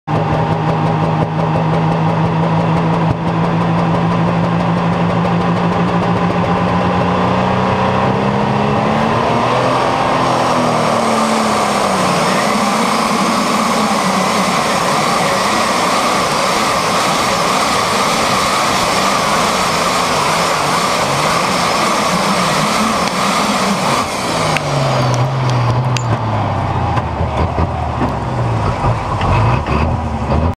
Outlaw tractor wheelies down the track!
Ride along with an Outlaw Pulling tractor at the Kossuth County Fair!